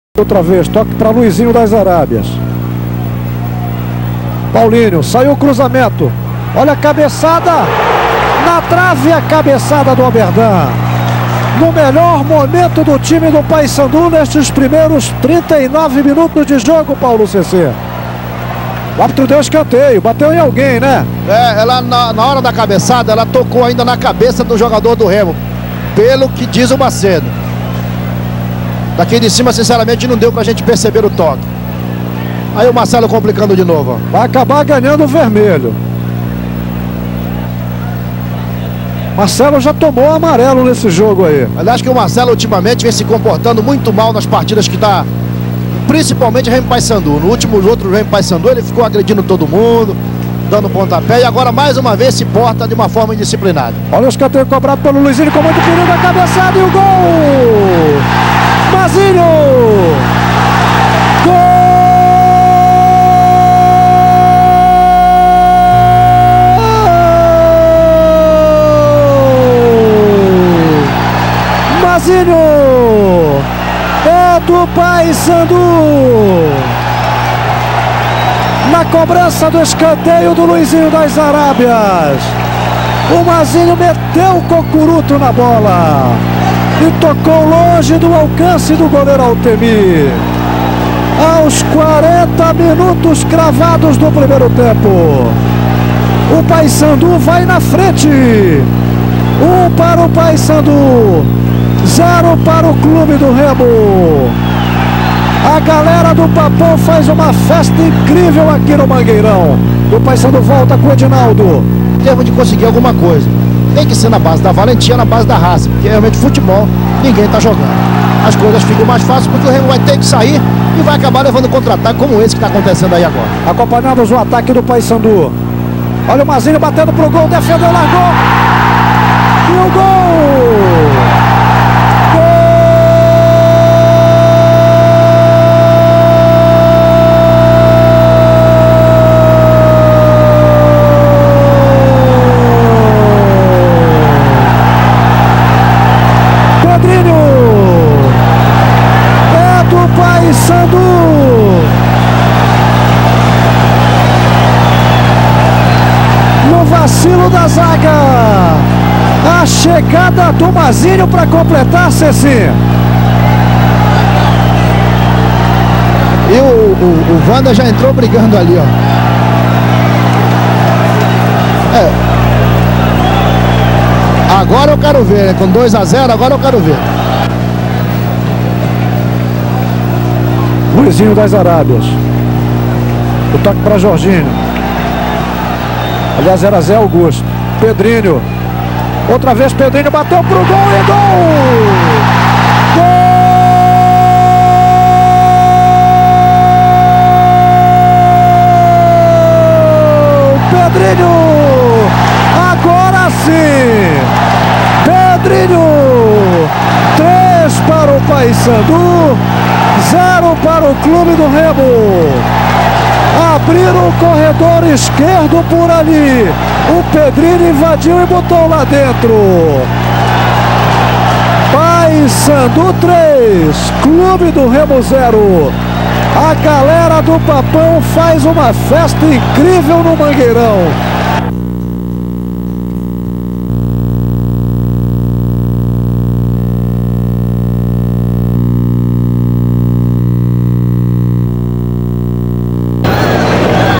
Paysandu 3 x 0 Remo - Campeonato Paraense 1988
RARIDADE !!!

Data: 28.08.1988
Local: Estádio Alacid da Silva Nunes (Mangueirão)
Árbitro: Antonio Macedo de Oliveira
Público: 15.396 pagantes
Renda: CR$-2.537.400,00
Gols: Mazinho, Oberdan e Pedrinho.
Cartões amarelos: Paulinho e Ademilton (PSC); Marcelo, Aleixo e Jorginho Macapá (CR).
Cartões vermelhos: Anésio (CR) e Nad (PSC)

Paysandu Sport Club: Samuel; Paulinho, Nad, Ademilton e Pedrinho; Zé Augusto, Oberdan e Mazinho; Edinaldo (Vânder), Luizinho das Arábias e Paulo Sérgio (Miguelzinho). Técnico: Givanildo Olivera

Clube do Remo: Altemir; Anézio, Ailton, Marcelo e Sânderson; Varela, Ondino e Bebeto (Juari); Aleixo, Rildon e Jorginho Macapá (Gleidston). Técnico: Dico.